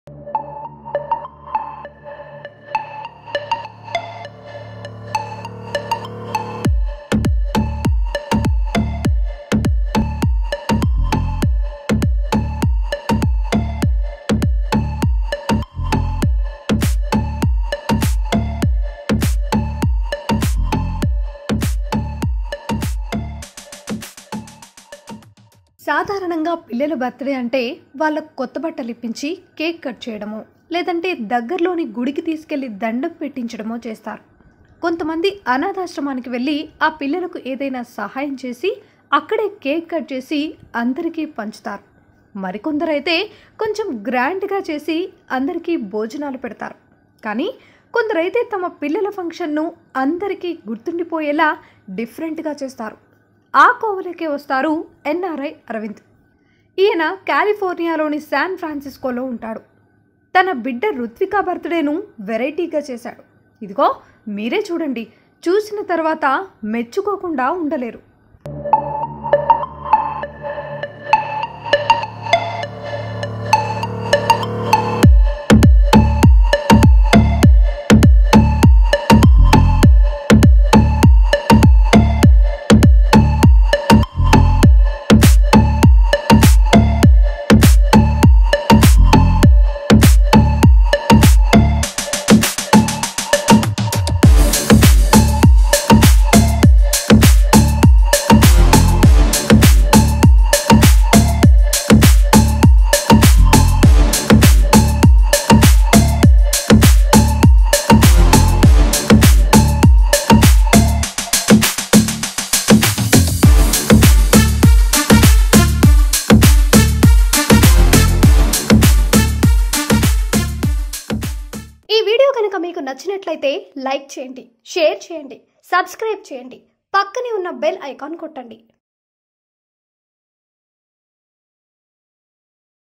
కూతురికి ప్రేమతో... నాన్న బర్త్‌డే గిఫ్ట్‌ అదుర్స్‌
సాధారణంగా పిల్లల బర్త్‌డే అంటే వాళ్లకు కొత్త బట్టలు ఇప్పించి కేక్‌ కట్‌ చేయమో లేదంటే దగ్గర్లోని గుడికి తీసుకెళ్లి దండం పెట్టించడమో చేస్తారు. కొంత మంది అనాథ ఆశ్రమానికి వెళ్లి ఆ పిల్లలకు ఏదైనా సహాయం చేసి అక్కడే కేక్‌ కట్‌ చేసి అందరికీ పంచుతారు. మరికొందరైతే కొంచెం గ్రాండ్‌గా చేసి అందరికీ భోజనాలు పెడతారు. కానీ కొందరైతే తమ పిల్లల ఫంక్షన్‌ను అందరికీ గుర్తుండిపోయేలా డిఫరెంట్‌గా చేస్తారు. ఆ కోవలోకే వస్తారు ఎన్‌ఆర్‌ఐ అరవింద్‌. ఈయన క్యాలిఫోర్నియాలోని శాన్‌ఫ్రాన్సిస్కోలో ఉంటడు. తన బిడ్డ రుత్విక బర్ద్‌డేను వెరైటీగా చేశాడు. ఇదిగో మీరే చూడండి.. చూసిన తరువాత మెచ్చుకోకుండా ఉండలేరు.